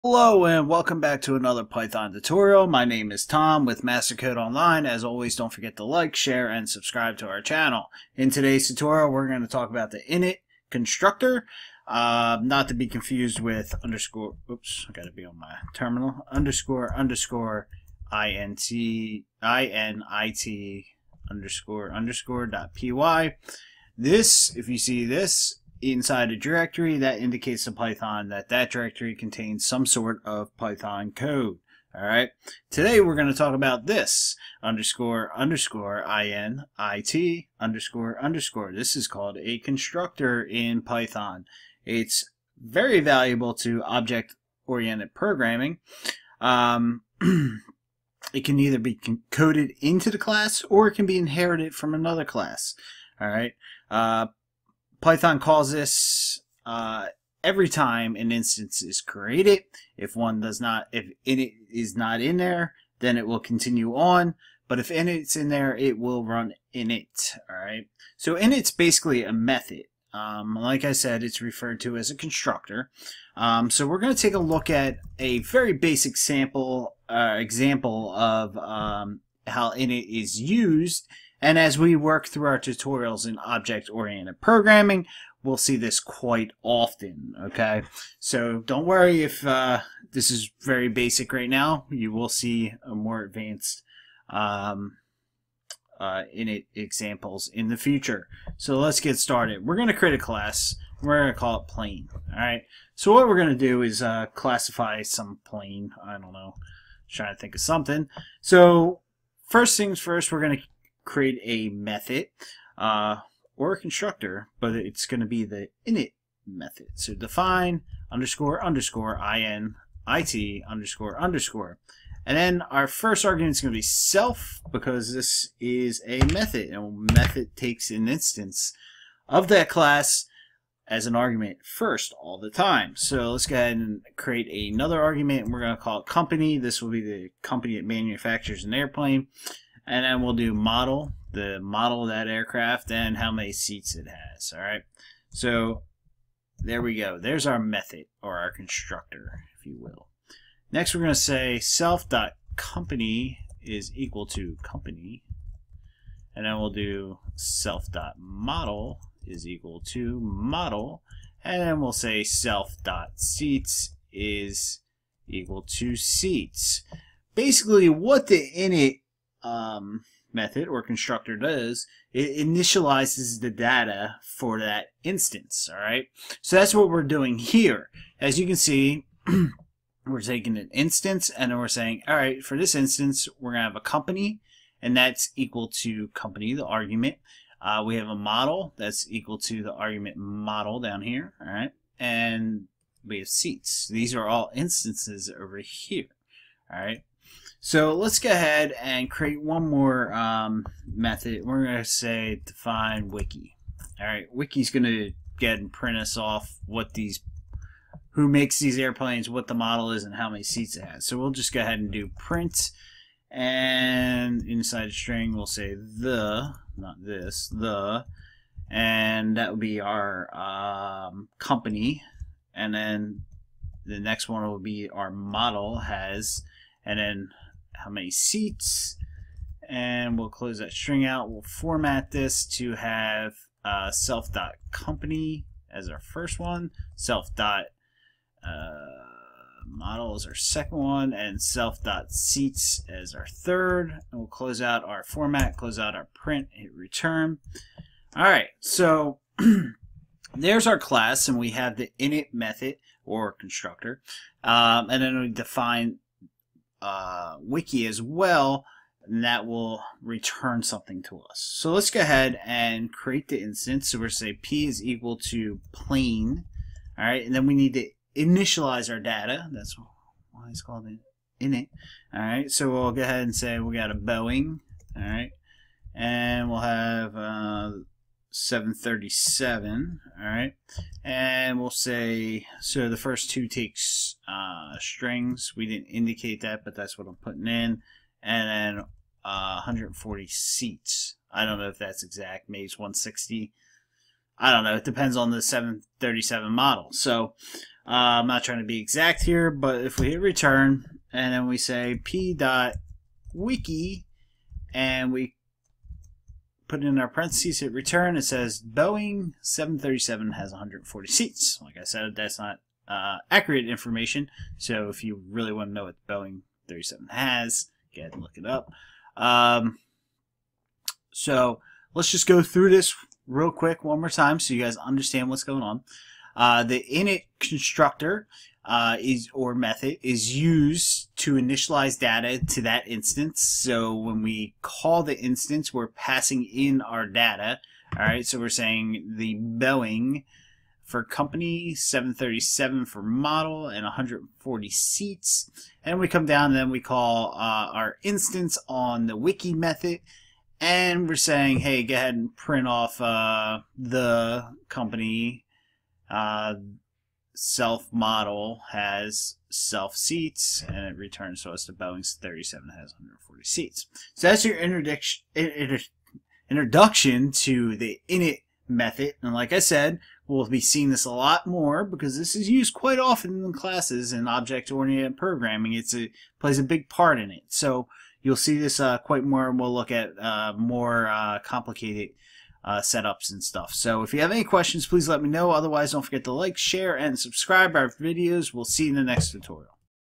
Python 3 Tutorial: Introduction To __init__() Constructor
In this Python 3 tutorial, we are going to look at the __init__() constructor in Python 3.